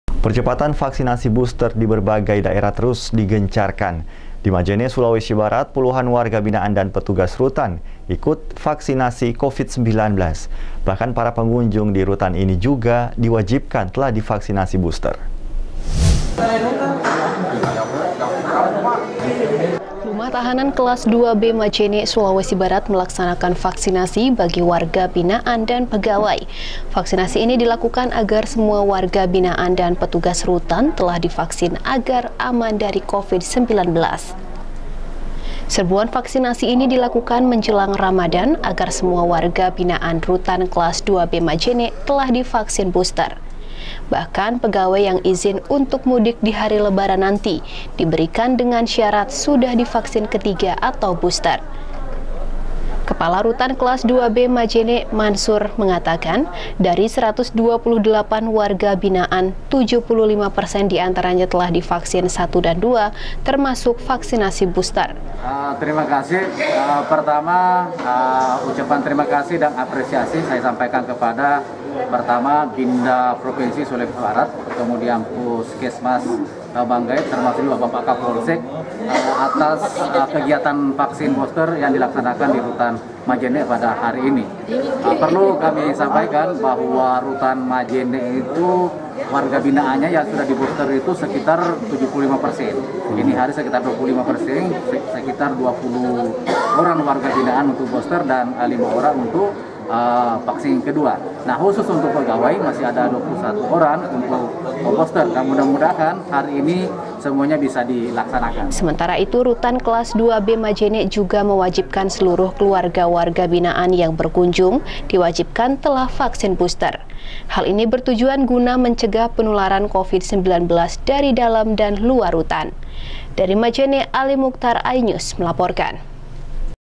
Vaksin Warga Binaan Rutan | iNews Sulbar | 29-03-2022
Percepatan vaksinasi booster di berbagai daerah terus digencarkan. Di majene,/ sulawesi barat, puluhan warga binaan dan petugas rutan, ikut vaksinasi covid-19. Bahkan para pengunjung di rutan ini juga diwajibkan telah divaksinasi booster.
Kontributor : Alimukhtar